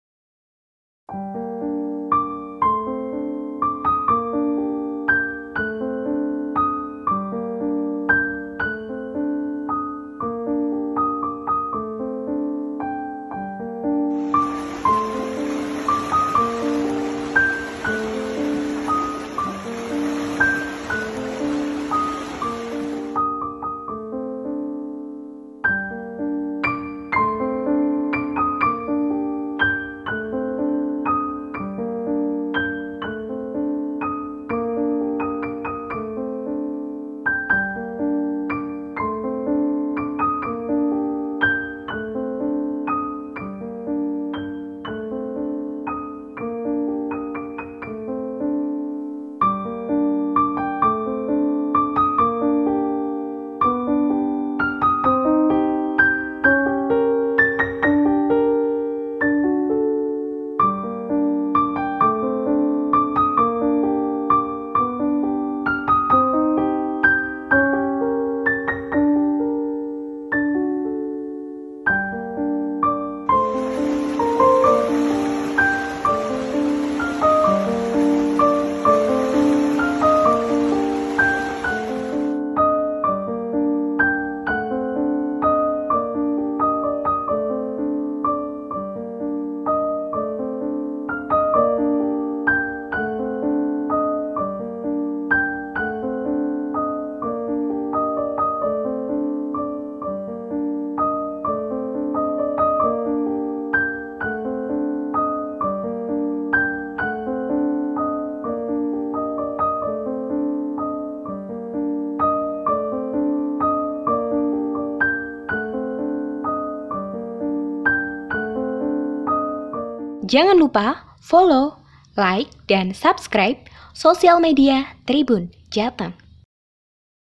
Viral Ridwan Kamil Lantunkan Azan di Pinggir Sungai Aare Sebelum Pulang ke Indonesia
Momen haru Gubernur Jawa Barat, Ridwan Kamil saat melantunkan azan di pinggir Sungai Aare, Bern, Swis. Tangan kanan sang Gubernur menutup telinganya untuk melantunkan azan.